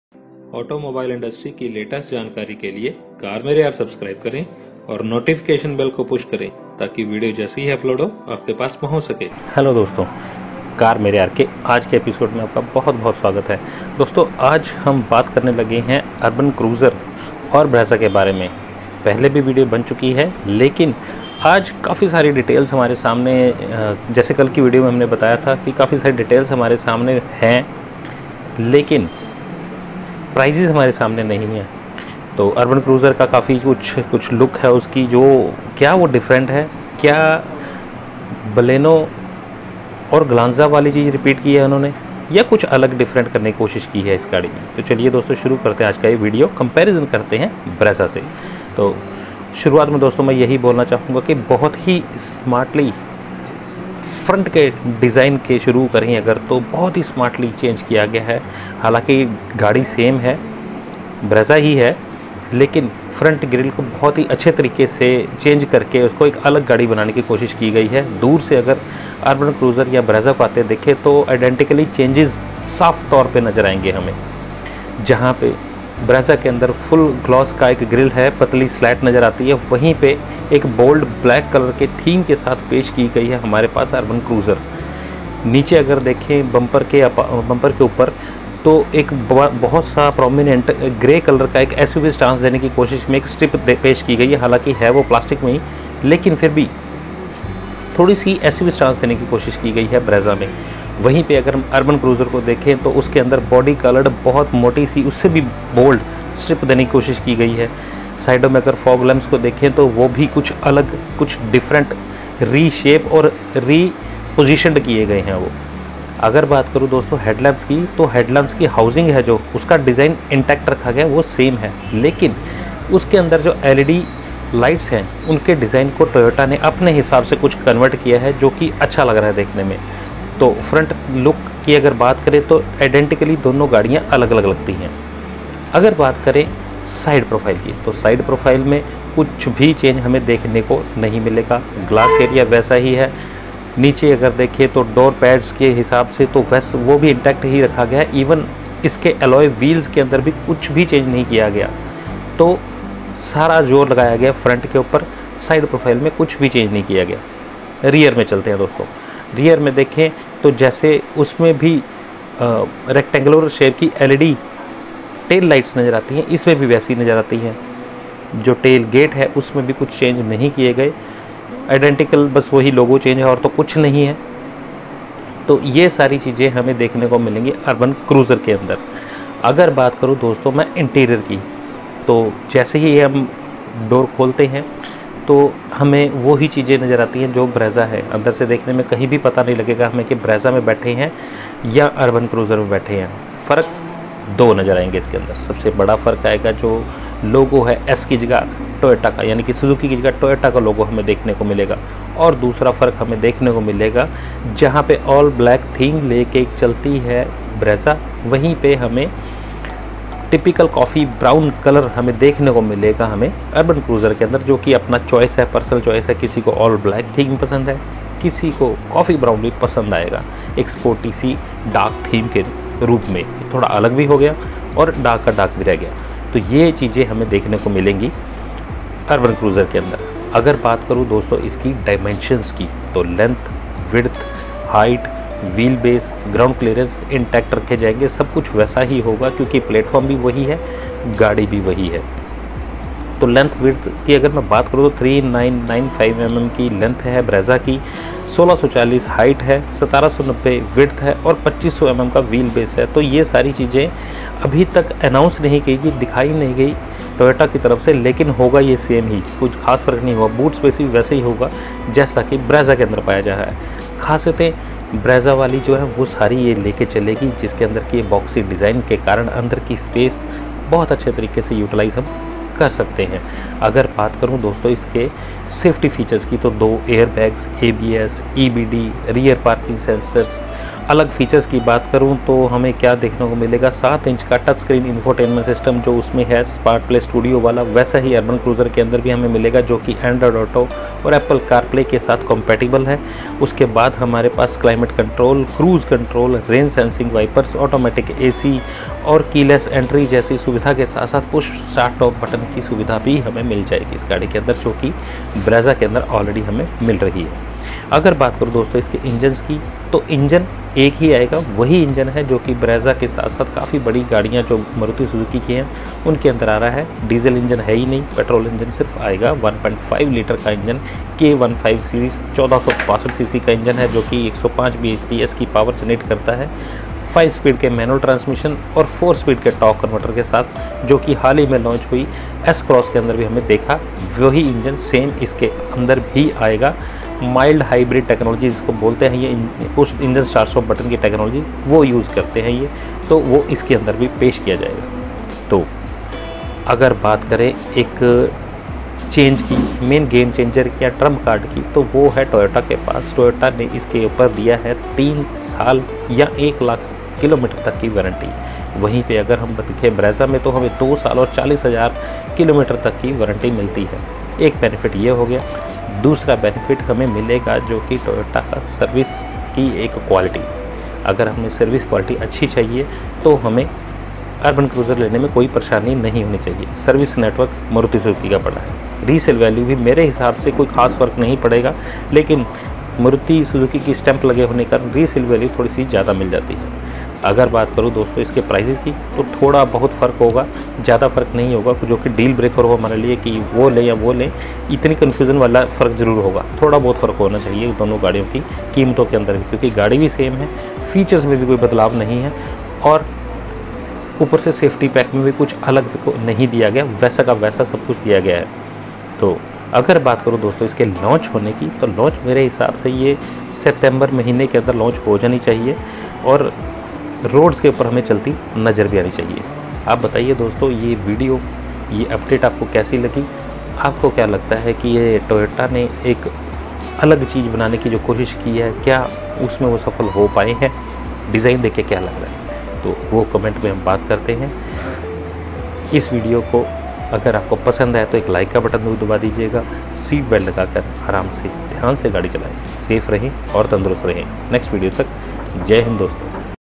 URBAN CRUISER VS BREZZA
long waited urband cruiser from toyota is finally here with all the interior and exterior looks, is it a copy of brezza like Glanza resembles same as Baleno, Let's check the details